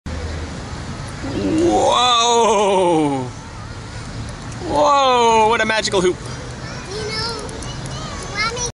Magic Floating Hoop
The kids discovered this effect the other day at the Square